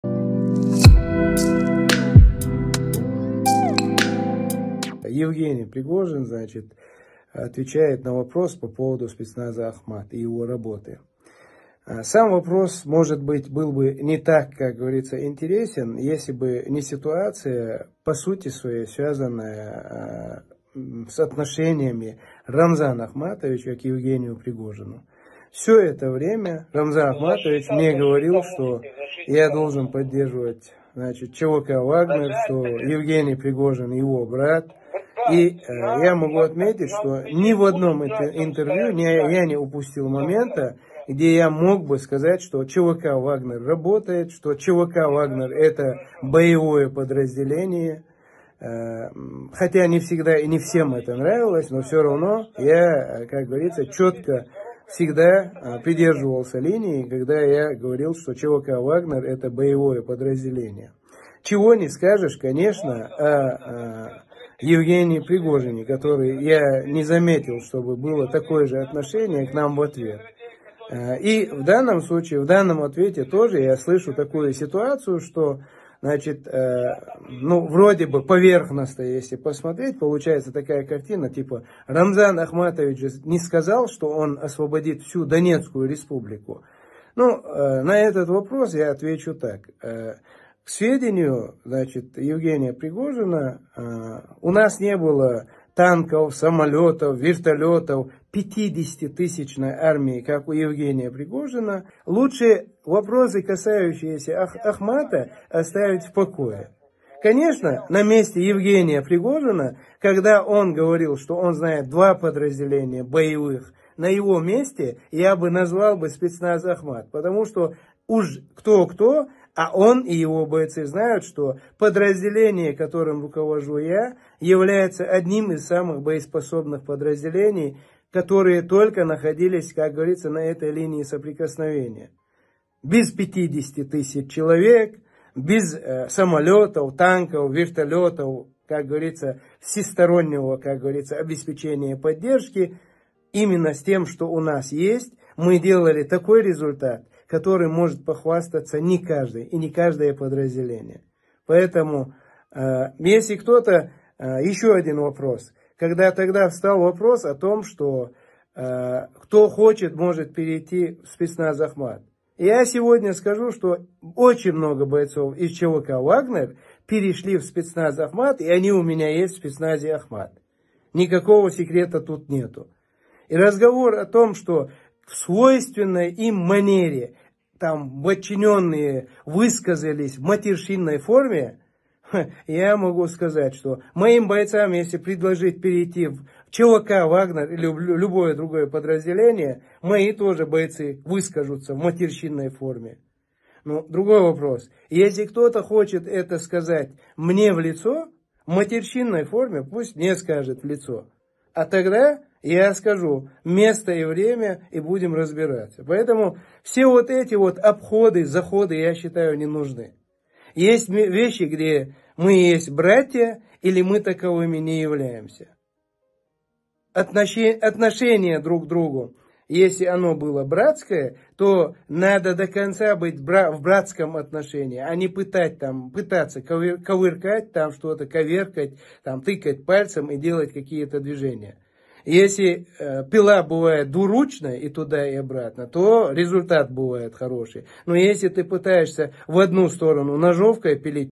Чеченский командир выступил против Пригожина. ЧВК Вагнера развалится?
Приветствую всех, с вами канал Мистерия. В этом видео поговорим про конфликт чеченских силовиков и Пригожина

Если у вас есть своё мнение по данному вопросу, то прошу отметить его в комментариях к данному видеоролику.